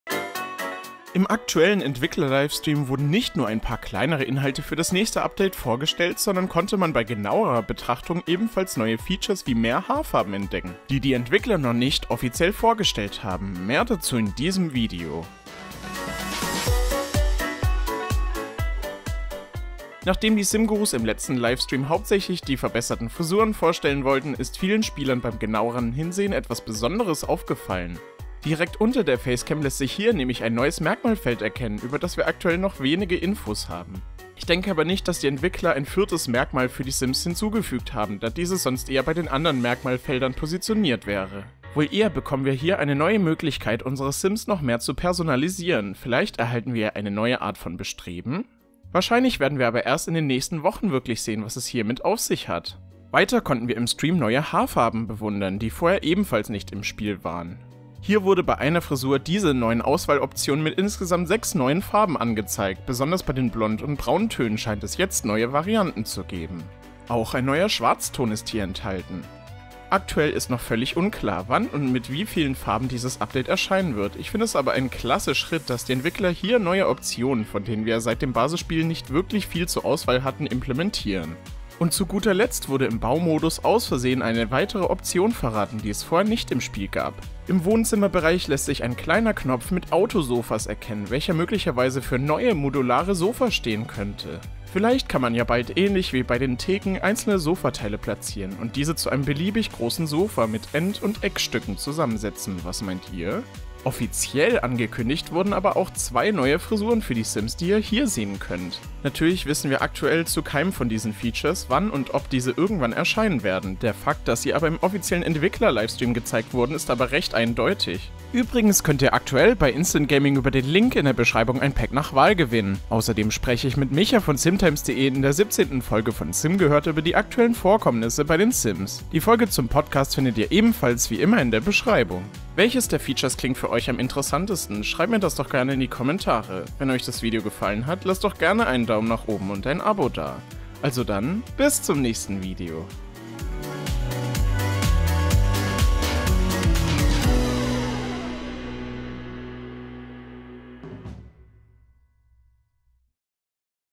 Neue Haarfarben, Frisuren & mehr für KOMMENDES Update! | Short-News | sims-blog.de
Im letzten Entwicklerstream wurden neue Features wie neue Haarfarben, sektionale Sofas und vieles mehr gezeigt, die ich euch in diesem Video zusammenfassen möchte.